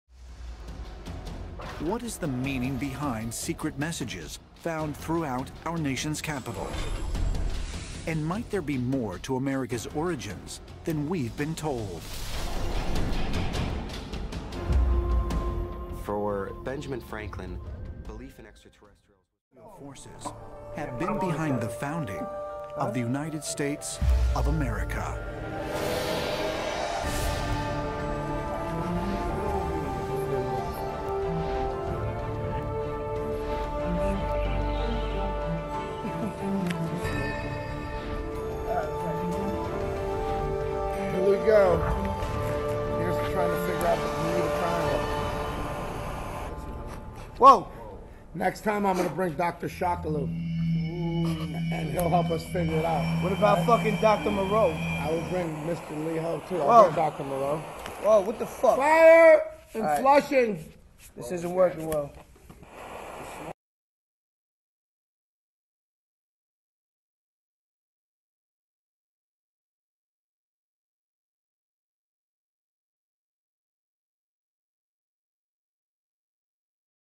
Action Bronson 360º blunt cam in JanusVR
A giant blunt was rolled around ricoh theta 360 camera and used as the skybox in a JanusVR world built from a scan of the green screen set for the Viceland series Action Bronson and friends Watch Ancient Aliens. This was the first experience Action had in VR and it was built in real time while the episode was being shot.